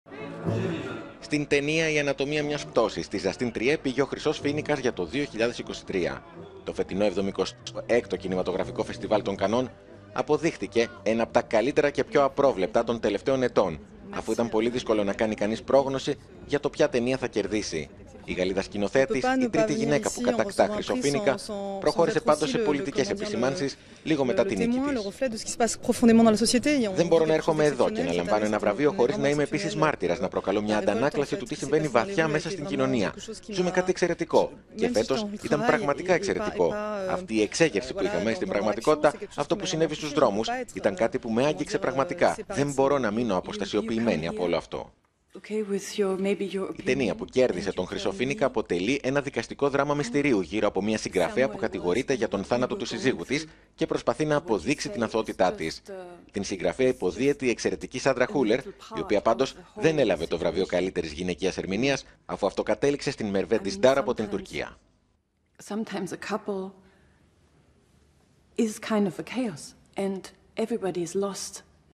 Φεστιβάλ Καννών 2023: Ο Χρυσός Φοίνικας στην «Ανατομία μίας πτώσης»
Εξαιρετικά ανταγωνιστικό φέτος το διάστημο φεστιβάλ